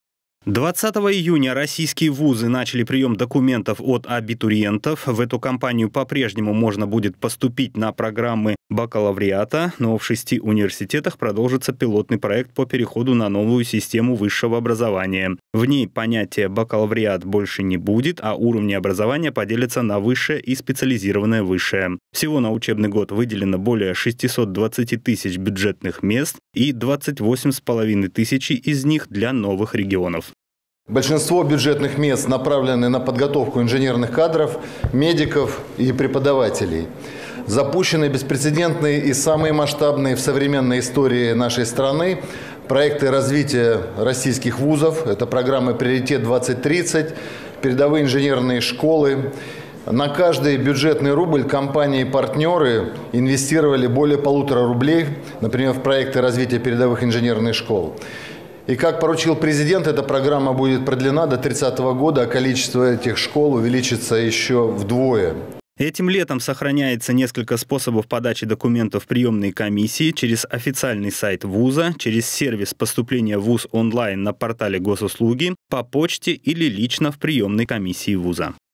РОССИЙСКИЕ ВУЗЫ НАЧАЛИ НАБОР АБИТУРИЕНТОВ
20 июня российские вузы начали прием документов от абитуриентов.